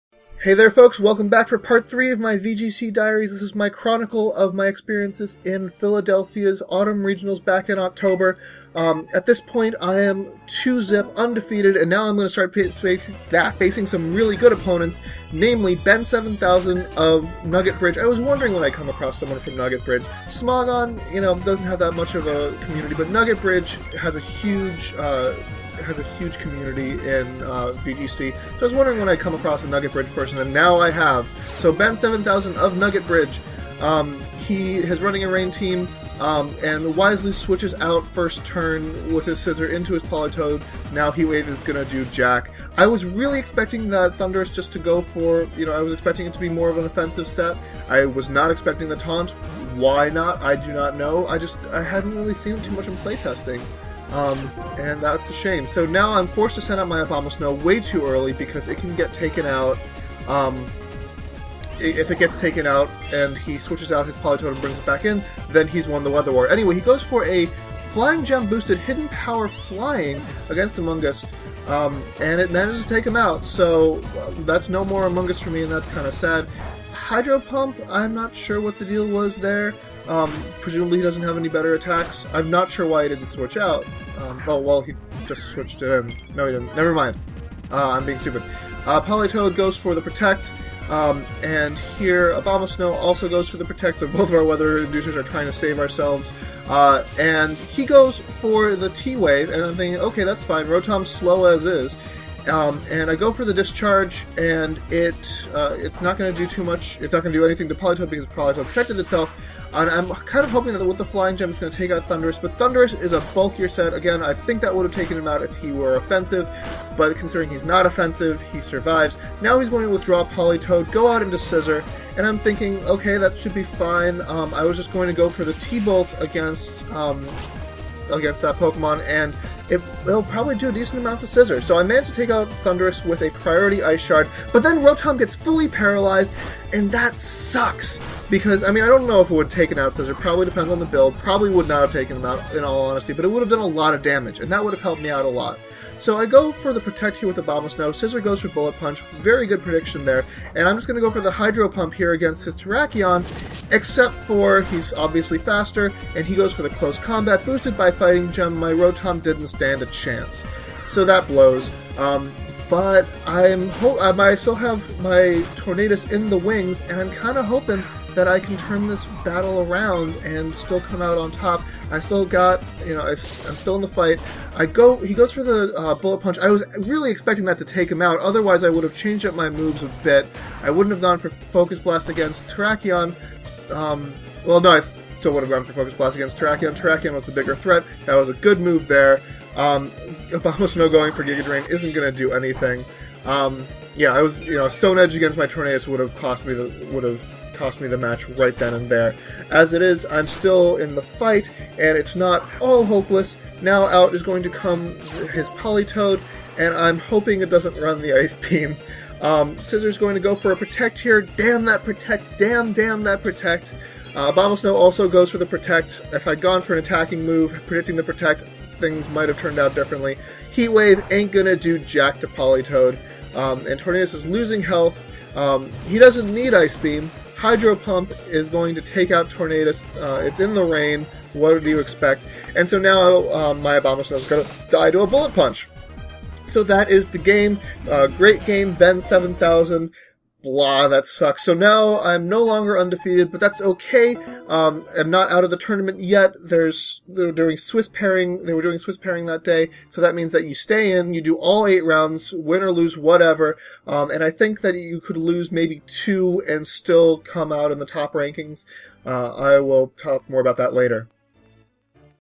VGC Diaries: Philadelphia Autumn Regionals: Round 3 vs. Ben7000 (Nugget Bridge)
Nugget Bridge is currently the premier English-language site for Nintendo formats (while Smogon focuses mostly on their own stuff). Consequently, Nugget Bridge players are usually regarded as among the best. I'd been wondering how long it'd take before I was paired with someone from Nugget Bridge (I'd been asking my opponents, but this dude's tee shirt was kind of a dead giveaway).

Anyway, Ben here finished the day with a 6-2 record, putting him in the top 16 (14th place, to be specific).